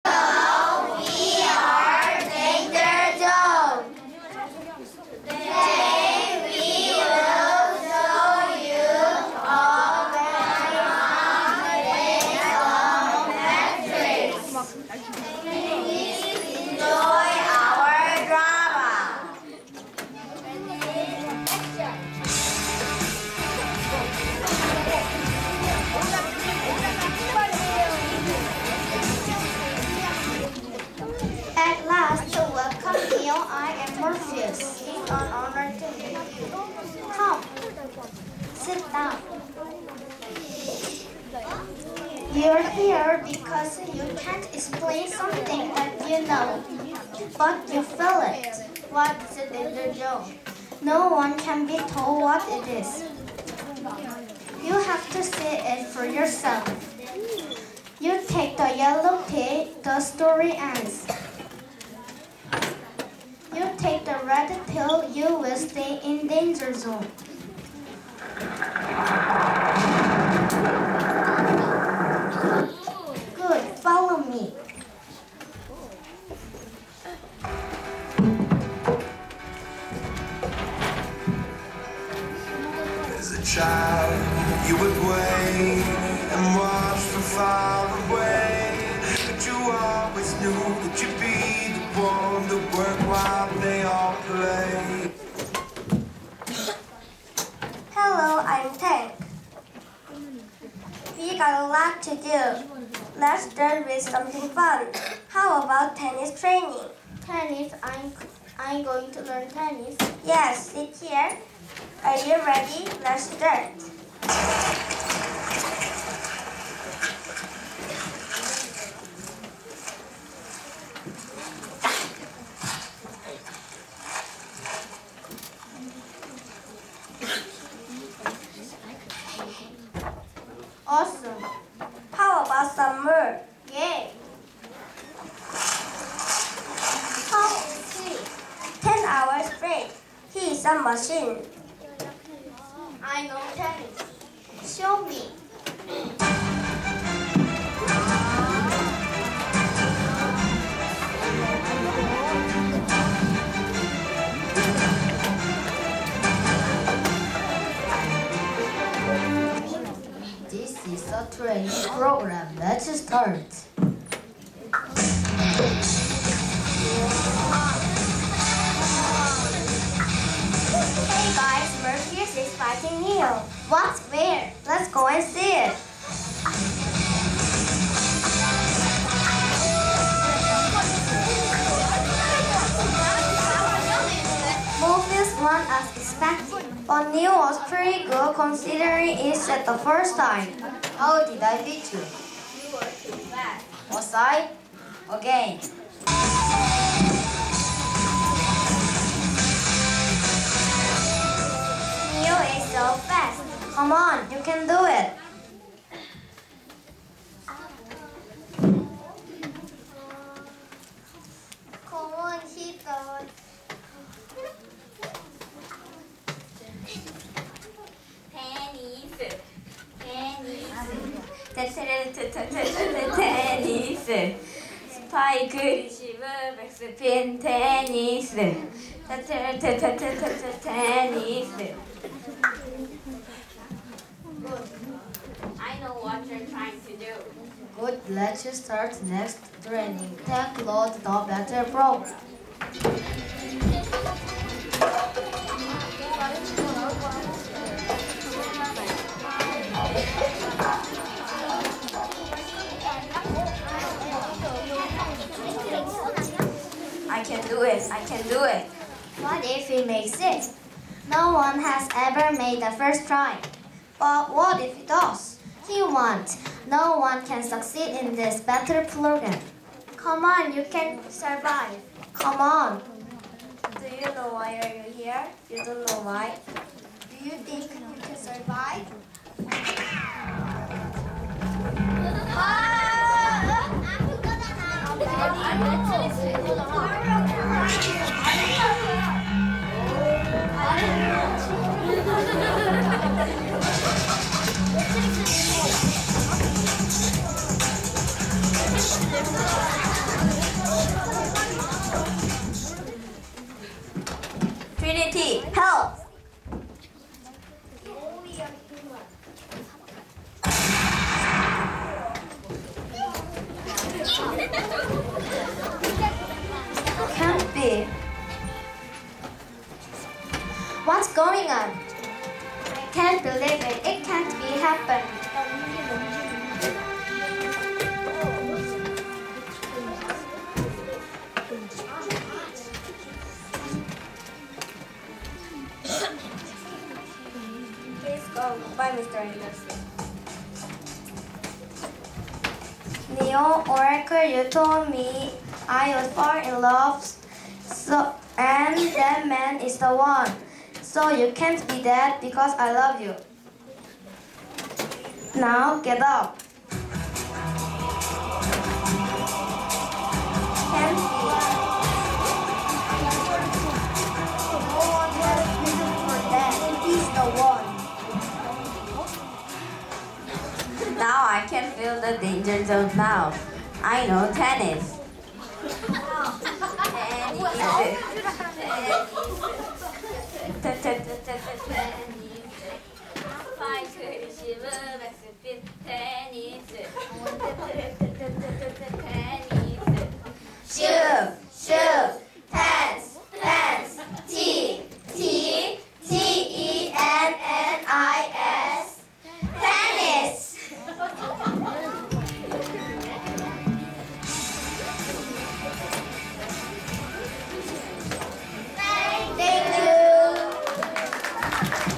2014 겨울영어캠프 마지막날 presentation_Danger Zone
Matrix를 바탕으로 한 드라마입니다. Danger Zone에 대하여 알고 싶어 하는 Neo가 그것을 찾기 위해 여러 가지 훈련을 받게 되고, 위기에 처하게 됩니다. Neo는 Danger Zone에 대하여 알게 될까요? Danger Zone반의 마지막 날 공연 동영상입니다.